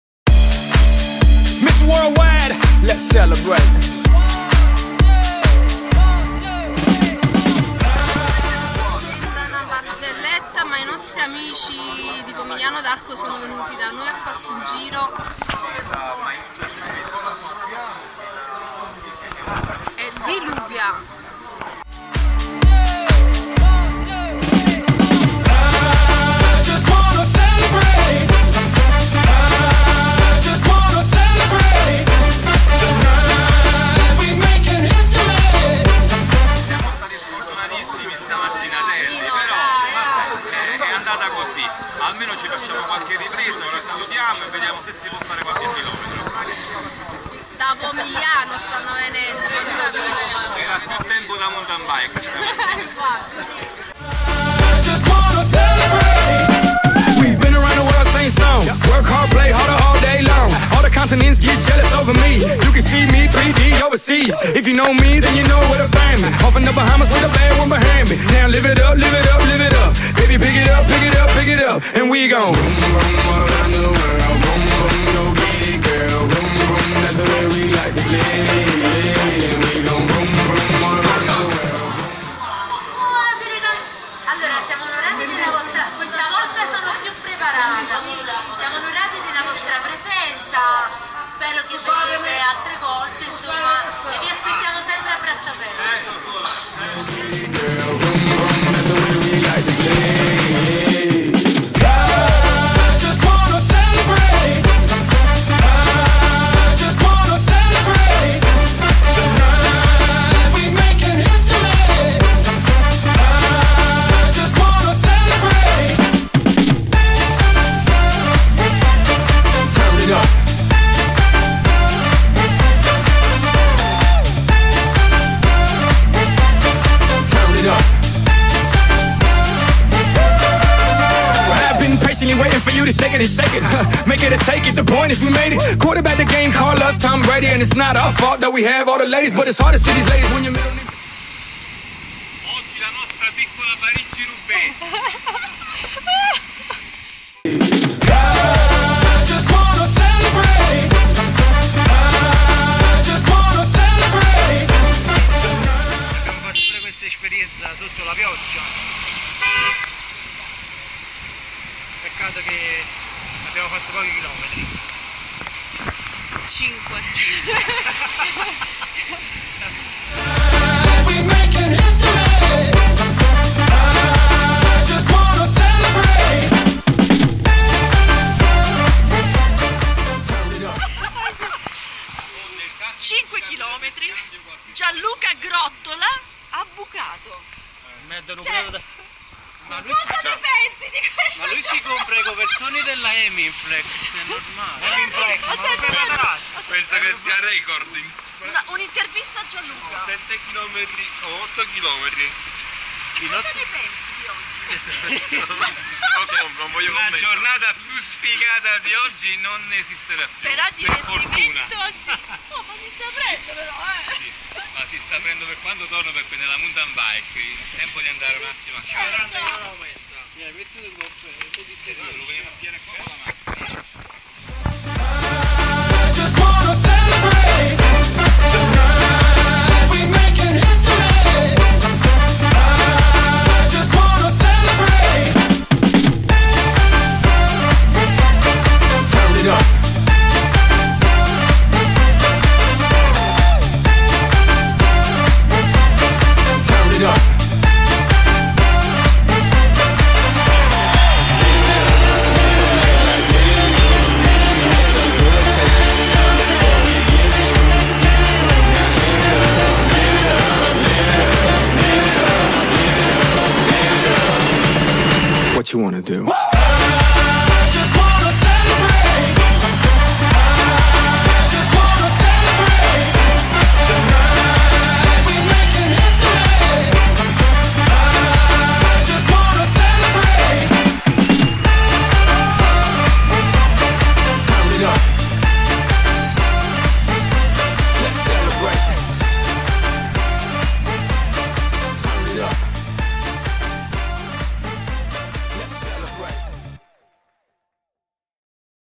Gemellaggio Ciclistico, Pomigliano d'Arco - Scafati.
La giornata della sfiga ... pioggia quando invece portava sole, solo 8 km percorsi e una bella bucatura il tempo di qualche foto ricordo, un caffè e tutti a casaaaaaa -_-